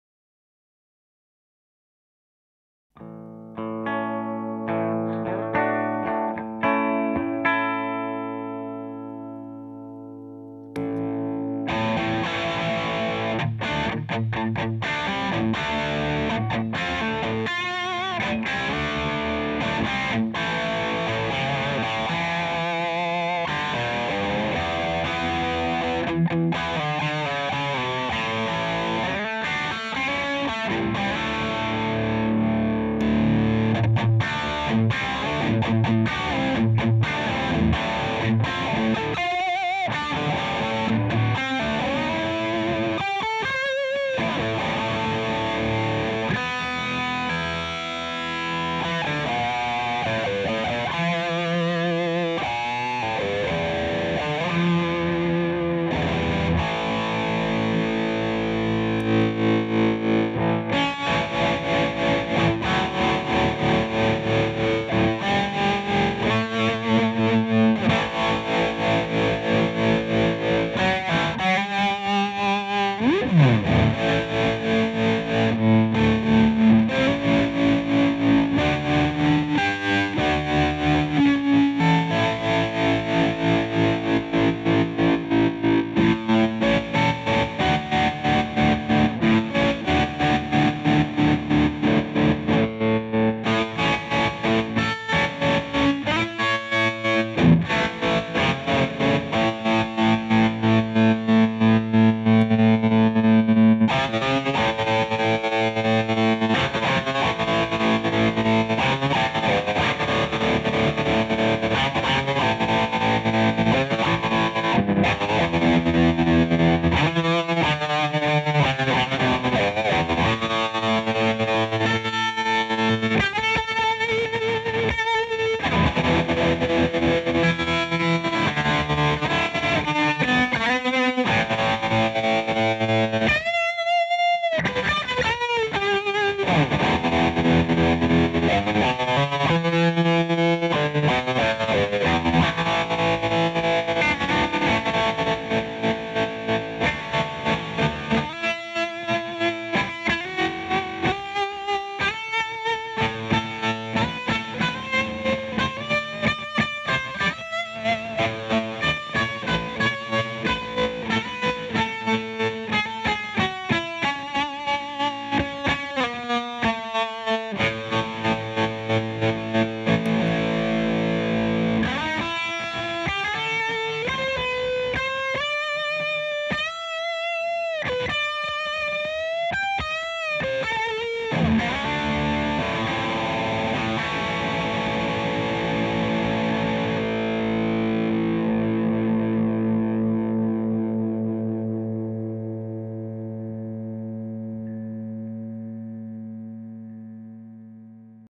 Dedalo Kamikaze y Tres - Distorsion y Tremolo - Handmade Effects
DEDALO FX

Estos son algunos de los sonidos que se pueden obtener combinando nuestro pedal Kamikaze KAM-3 Distortion (en este caso versión Stencil 2012) con el Tres TRS-3 Tremolo.

Ambos pedales son True Bypass, fabricados en Buenos Aires, Argentina.

These are some of the sounds you can obtain by combining our pedal Kamikaze (KAM-3 Distortion) with the TRES (TRS-3 Tremolo). The Kamikaze belongs to our 2012 line of Stencil pedals.

Both pedals are True Bypass and handmade in Buenos Aires, Argentina.